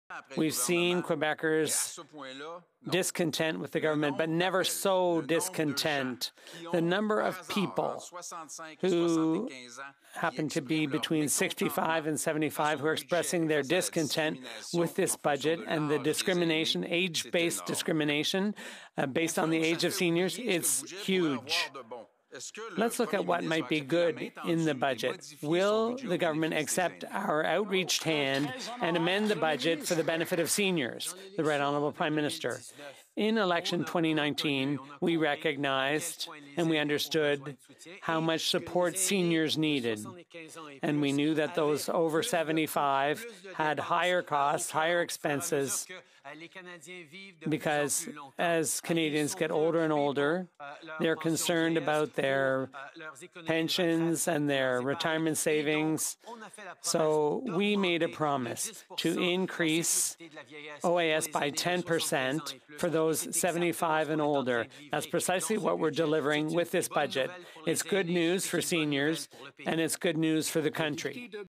Blanchet claims that Trudeau's 2021 federal budget is 'age-based discrimination'
Bloc Quebecois Leader Yves-François Blanchet criticized the federal budget, claiming that it was 'age-based discrimination.'